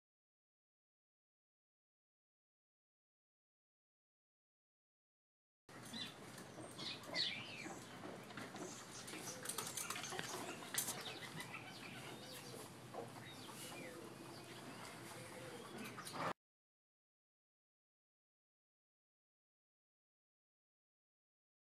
DTR video - 111006MMMa1.wmv
Mucho Macho Man was captured on this short clip of him getting ready to go to the track as he prepares for his return to racing following the Triple Crown races.